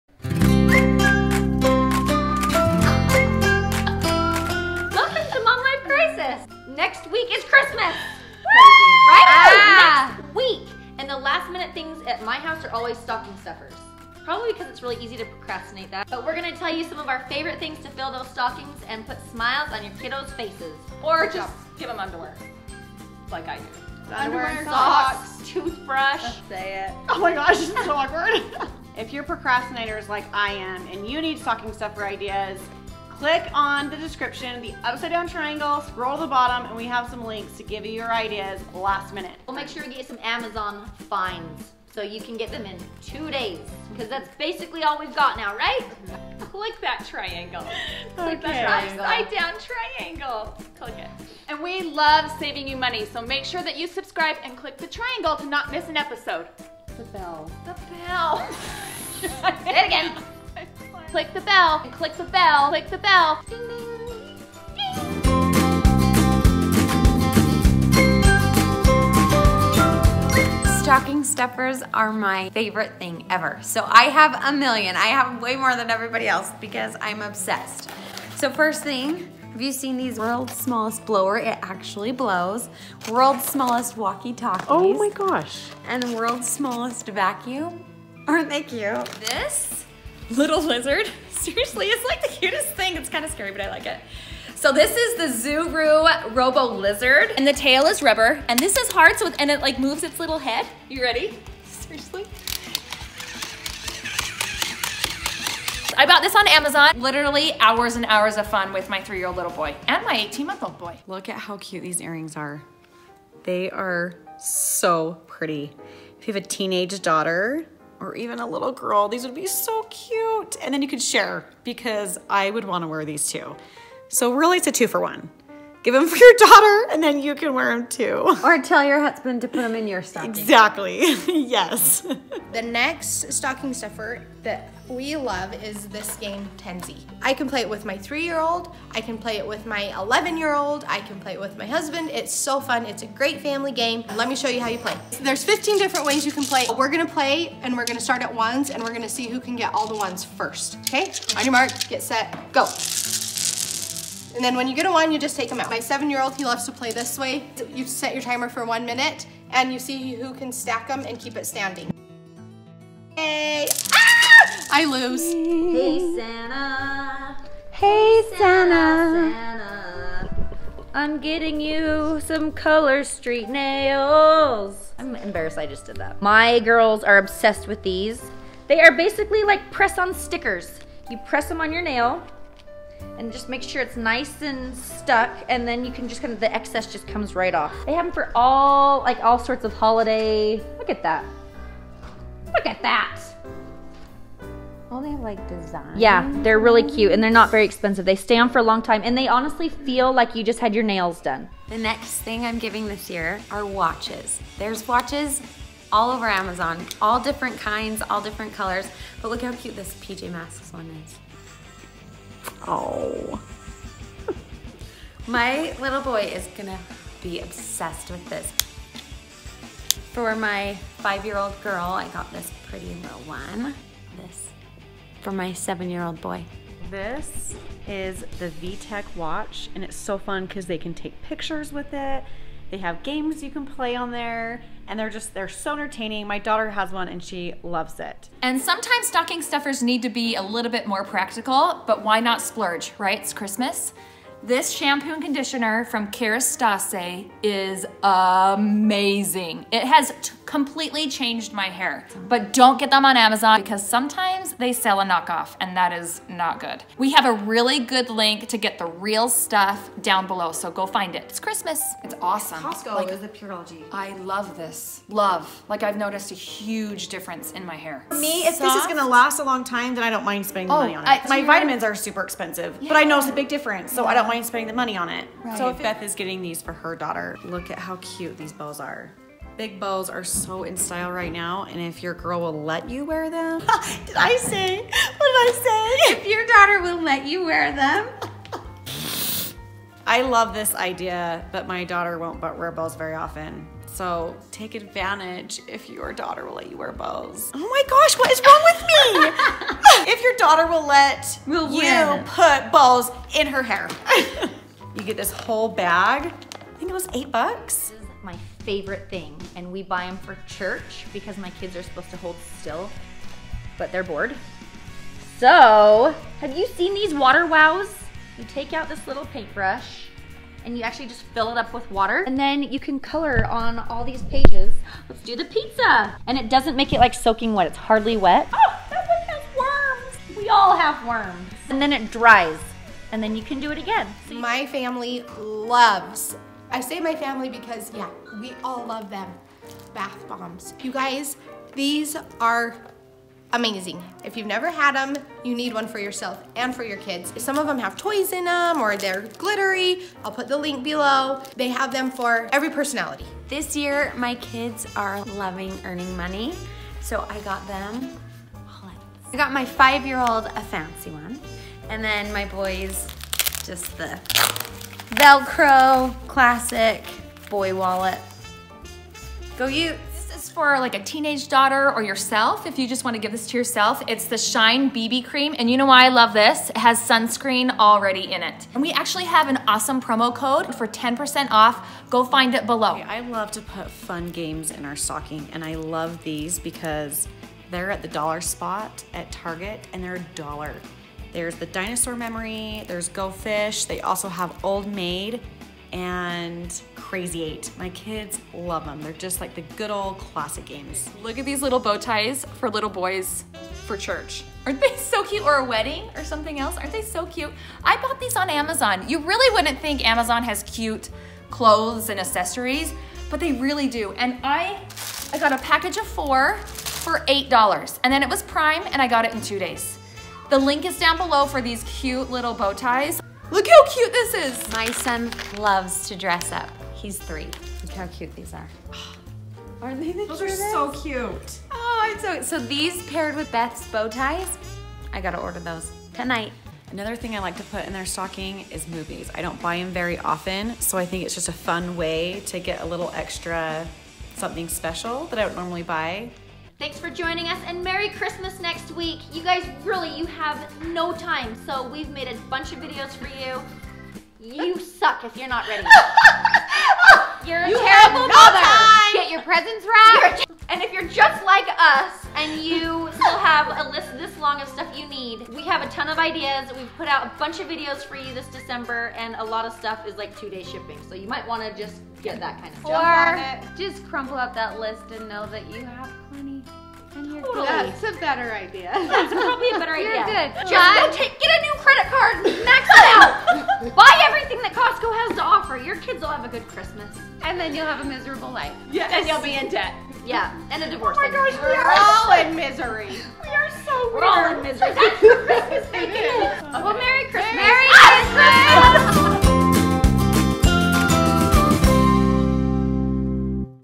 2019 BEST CHRISTMAS GIFTS FOR KIDS - STOCKING STUFFERS
Are you looking for the best Christmas kids gifts ideas of 2019? Hottest Christmas Toys 2019. We have researched and found you the very BEST KIDS CHRISTMAS GIFTS and top toys of 2019!!! We have the best Christmas gifts ideas for GIRLS and for BOYS! We found the most adorable baby stroller and fairy garden for girls! Have you thought about getting your kids a drone? We have the best drone with the BEST prices! You are searching for fun gifts for the family! We love Sleeping Queens card game and the Ping Pong Set that we can take anywhere! What about the ECHO DOT? Kids of all ages are loving the Amazon Echo Dot! Our boy moms are loving the remote control monster truck and of course Batman Legos. Do you have a reader? We love the A-Z mystery books. What about those creative geniuses? We found the PLAY MAGS, magformers! What about those TWEENS? Look no further. They will LOVE the Polaroid Instax Camera and Camera Case. We found the cutest Construction Plate and Utensils for when you are needing to keep those toddlers busy! Don't worry MAMA's! Momlife Crisis has done all the work for you!
Oh wait, you thought we would forget about the gift wrapping but we didn't!!! WE FOUND THE BEST GIFT WRAP IDEA OUT THERE! We love RUBY SWAN HOLIDAY REUSABLE GIFT BAG! They come in 3 different sizes and in a pack of 5. They are the cutest HOLIDAY GIFT BAGS! We're thinking that SANTA might really like this idea!

Bath Bombs

World Smallest Blower

Worlds Smallest Walkie Talkies

Worlds Smallest Vacuum

Zuru Robo Lizard

Waterproof Boys Watch

Girls Flower Watch

PJ Mask Tech Watch

V Tech Watch

Girls Hair Bows

Girls Wallet

Utah Utes Wallet

Boys Bowties

Boys Suspenders

Incredibles 2

Melissa and Doug Water Wow